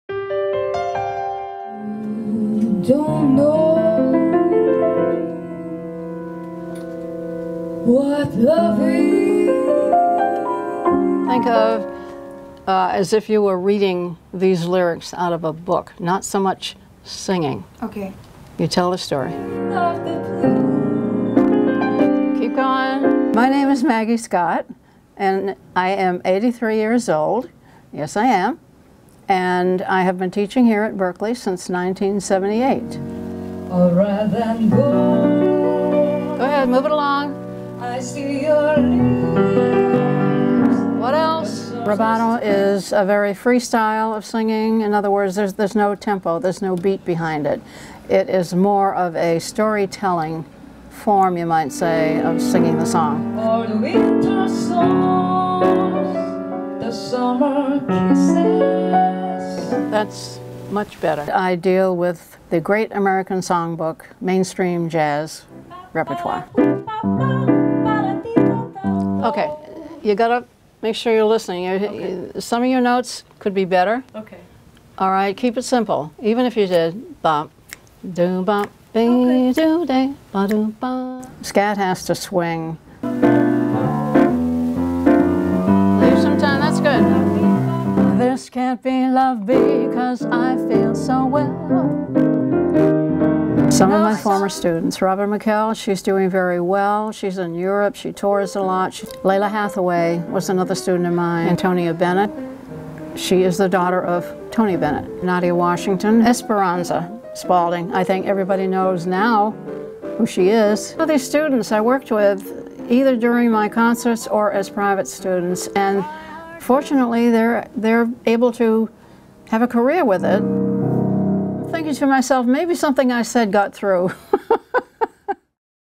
Berklee voice coach Maggie Scott shares her love of jazz
(The Boston Globe) Jazz pianist and vocalist Maggie Scott has instructed three decades of students at Berklee (Video by Wendy Maeda/Globe Staff, produced by Leanne Burden Seidel/Globe Staff)